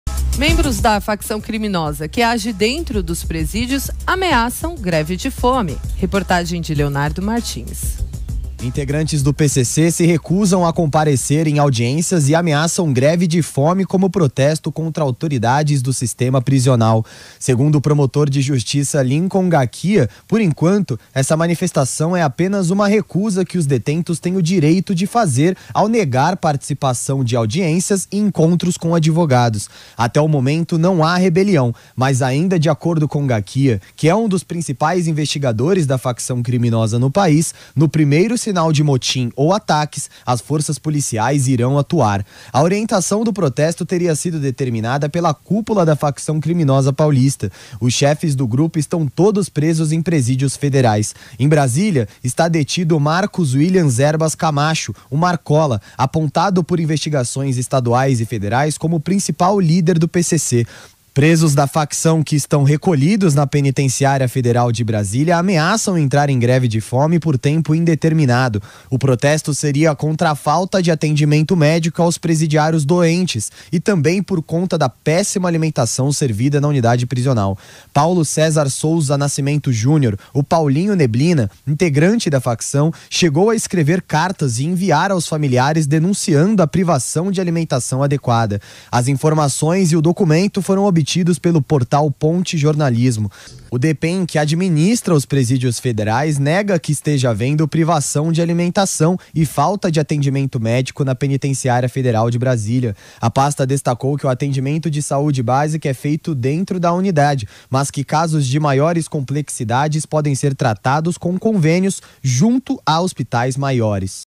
PCC, essa merda não é SPA! Ahhh, esses aliados do PT!
Membros do PCC ameaçam entrar em greve de fome. Uma das razões do protesto é a péssima alimentação nos presídios. Essa merda não é SPA, bando de vagabundo! Ahhh, esses aliados do PT!